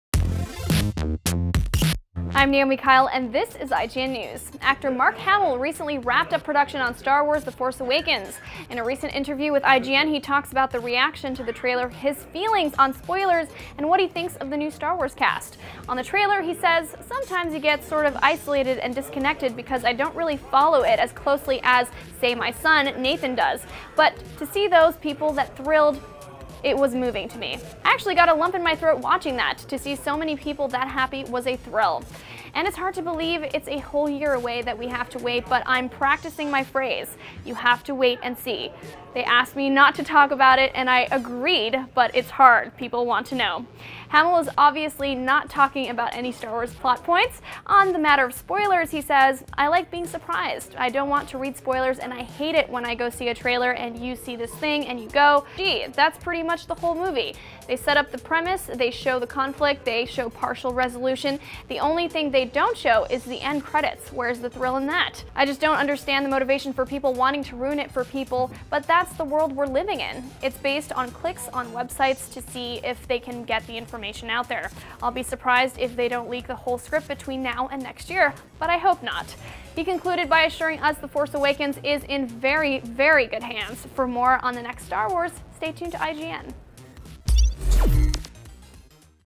Mark Hamill On the Star Wars Trailer & Spoilers - IGN News
This is a busy time for Mark Hamill. The iconic actor recently wrapped up production on Star Wars: The Force Awakens, and now it's been revealed he'll be guest starring on The Flash in the role he played in the 1990 TV version, as The Trickster.